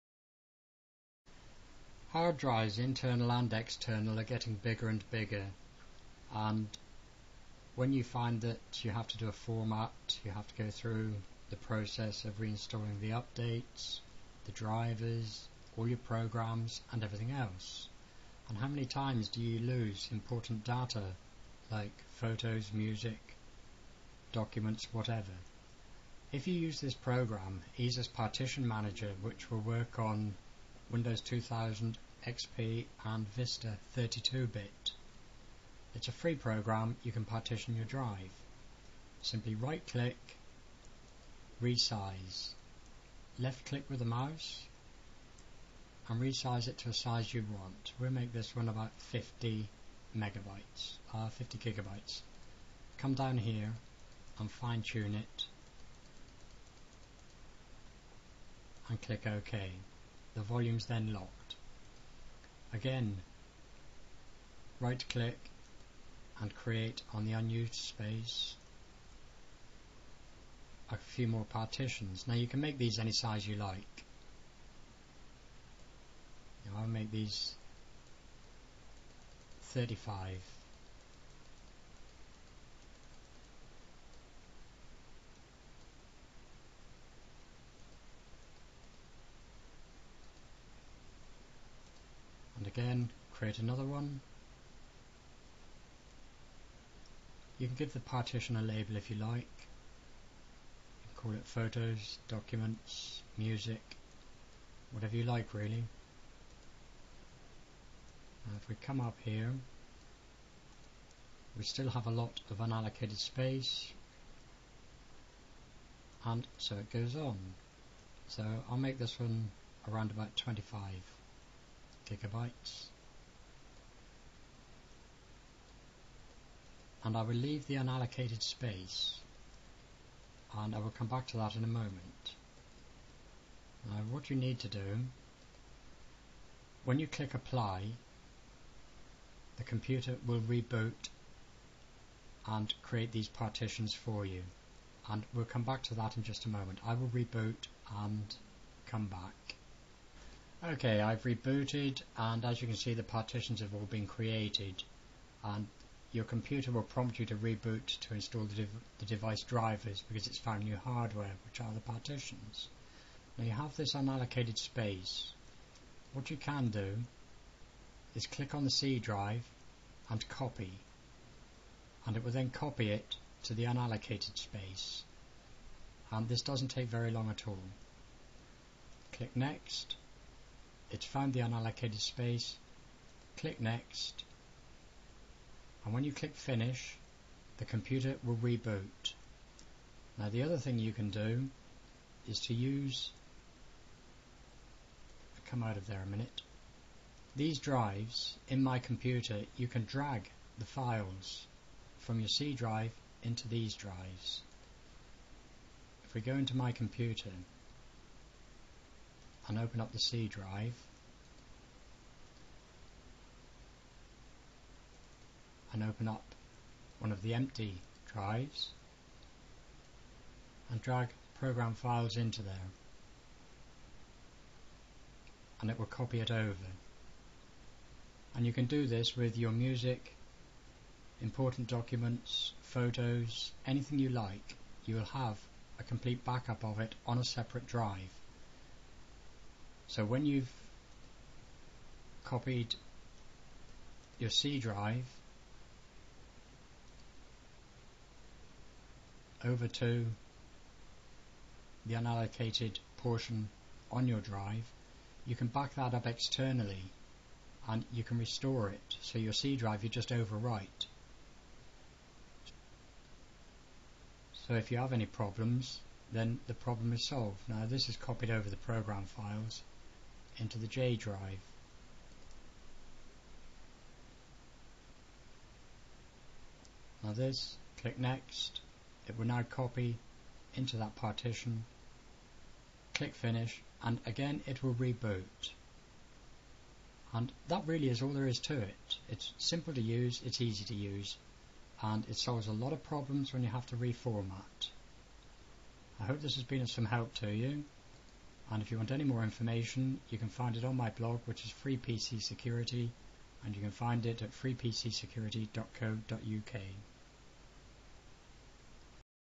Easeus Partition Manager Free
Easily partition your hard drive, copy your data and save files and folders in partitions. Need to format? Use the boot CD and replace operating system files.  Works on Windows 2000 / XP / Vista (32bit only)